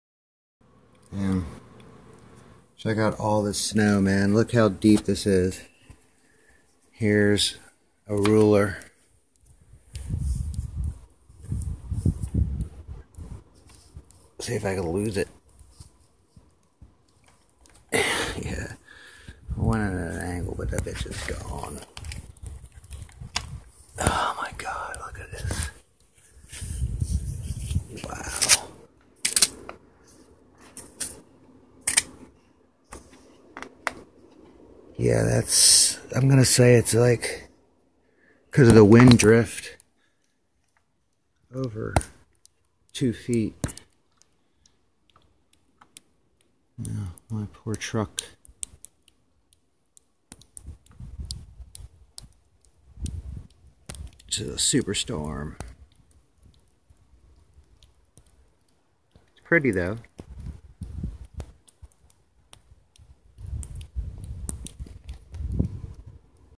Winter Snow Storm Jonas in Maryland 1-23-16
Some cellphone footage of winter storm Jonas. Packed in tight.